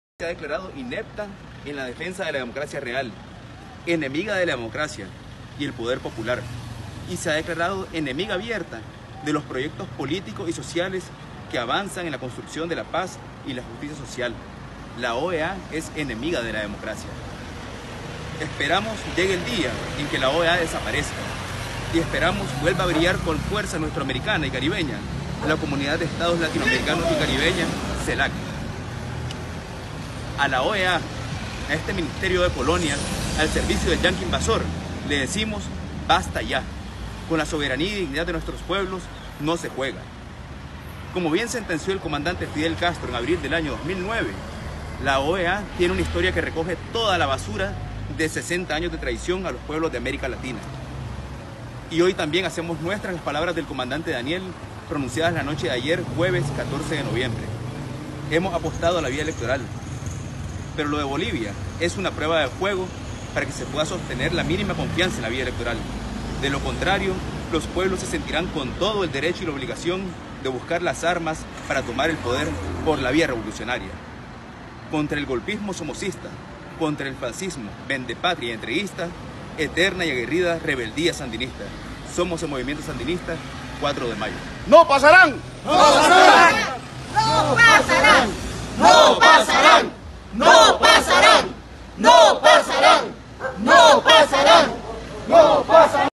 Juan Carlos Ortega llega a despotricar frente a la OEA en Nicaragua
Es su segunda aparición pública con su grupito de amigos. Primero fue para llegar a amenazar al Consejo Superior de la Empresa Privada,  ahora llegó hasta la sede de la Organización de Estados Americanos (OEA) a pedir la desaparición del organismo continental. Ortega Murillo parece encampañado para perfilarse como el sucesor de sus padres.